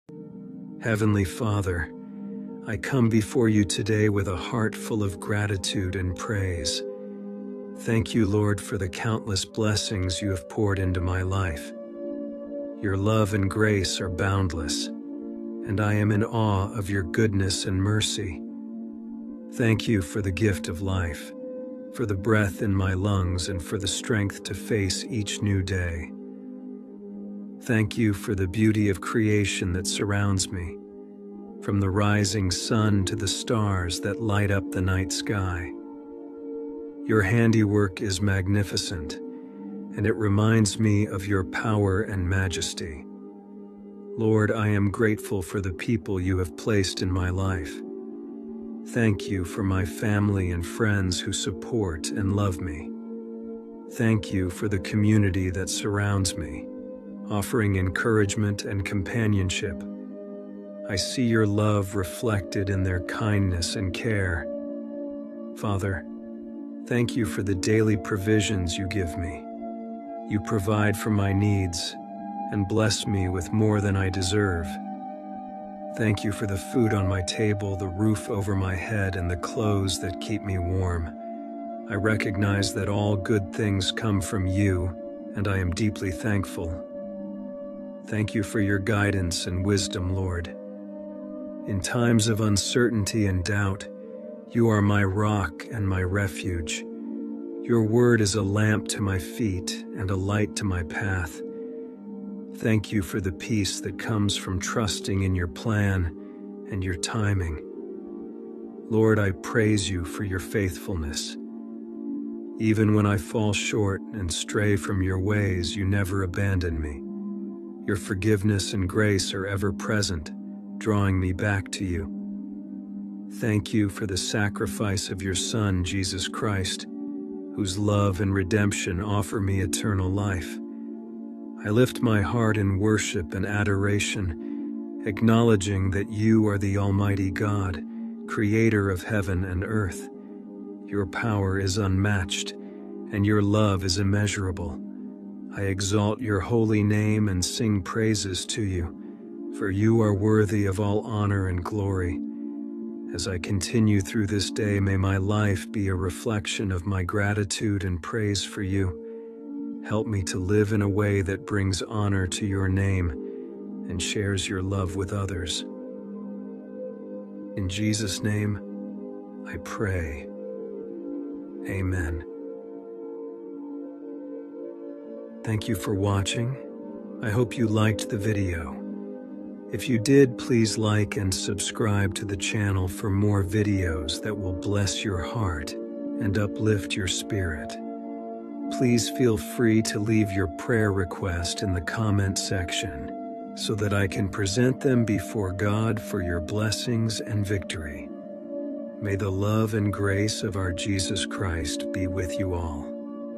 Lord God, Thank You for the daily provisions You give me. You provide for my needs and bless me
a simple prayer to thank god for his blessings
A short prayer to god
morning prayer
night prayer
evening prayer
catholic
powerful prayer